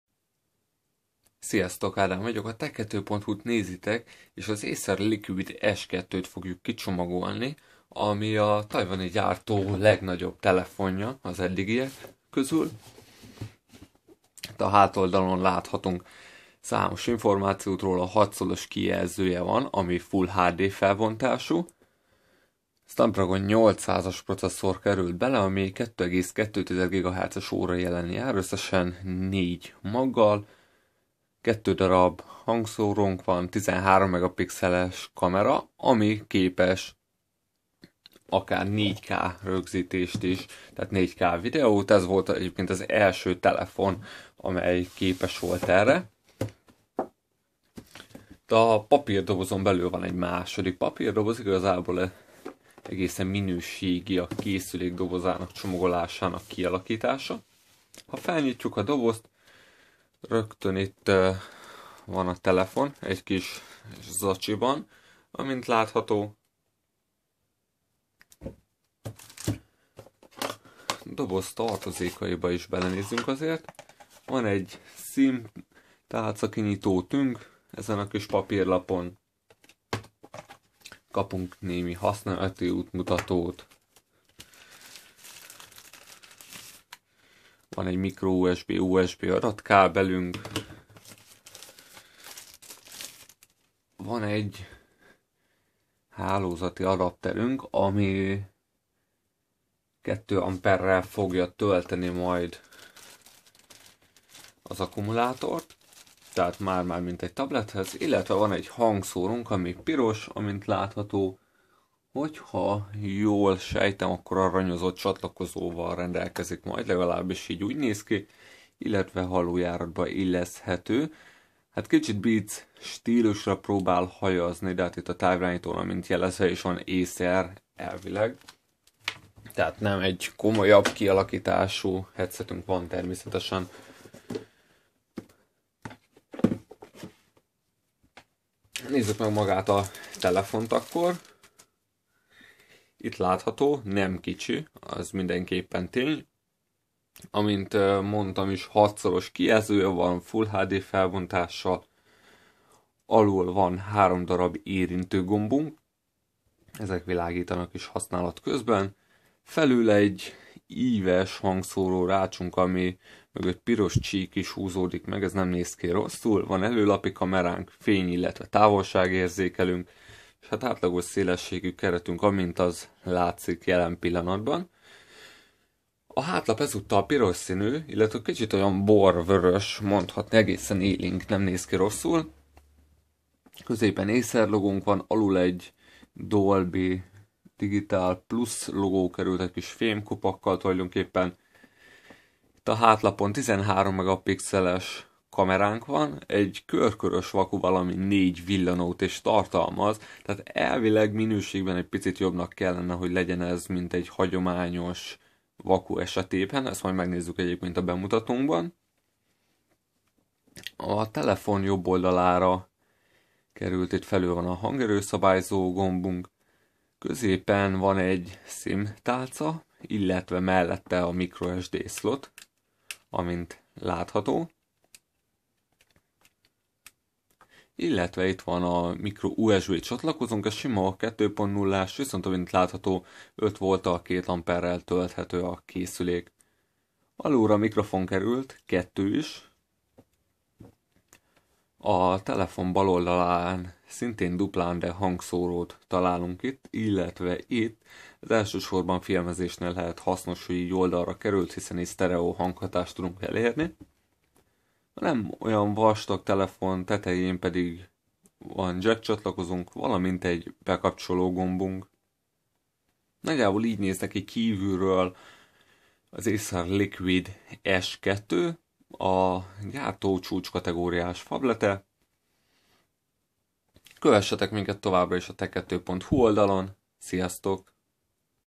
Acer Liquid S2 okostelefon kicsomagoló videó | Tech2.hu
Kicsomagoltuk a piros színű hátlappal, 6"-os FullHD IPS kijelzővel és Qualcomm Snapdragon 800-as processzorral rendelkező Acer Liquid S2 modellt.